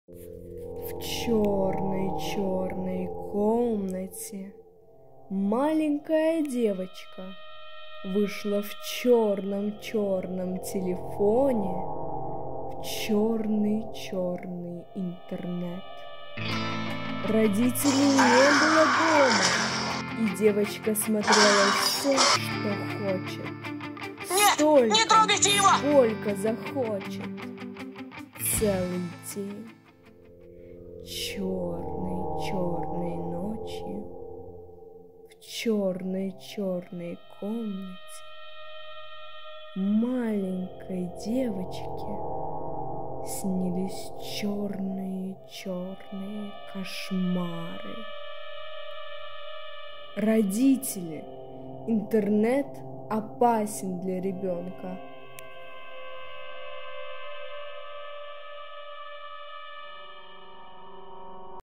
Маленькая девочка заблудилась в интернете
«Сестрорецкое болото  - пейзаж, которому тысяча лет»
Баукина Рада Владимировна,  6 лет
г. Санкт-Петербург